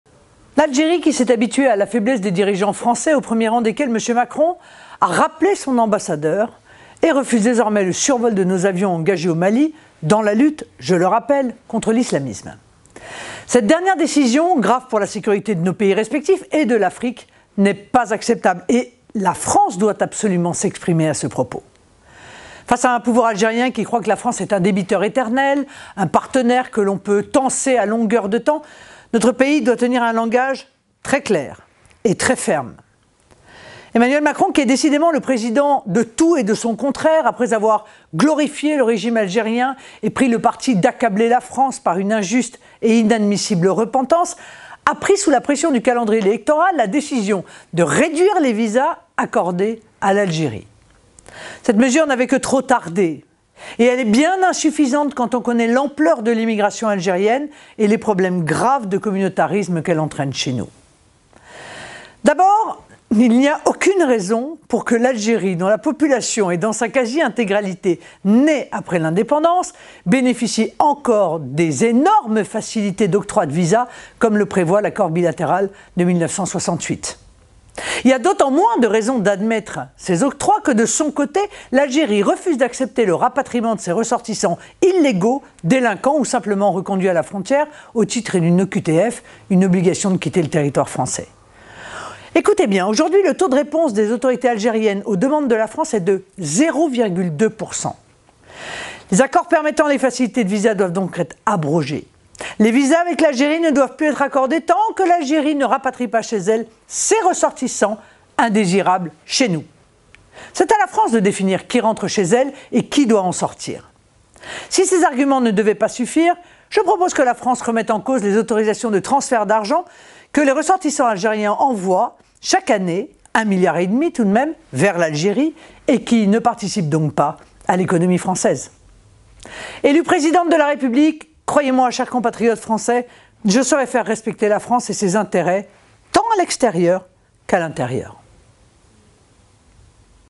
Face à l'Algérie, il faut faire respecter la France !
C'est à la France de définir qui peut rentrer chez elle et qui doit en sortir.

Fin des facilités de visas, blocage des transferts d'argent de la diaspora : nous avons les moyens d'agir face à l'Algérie et à son refus de récupérer ceux de ses ressortissants qui sont indésirables chez nous.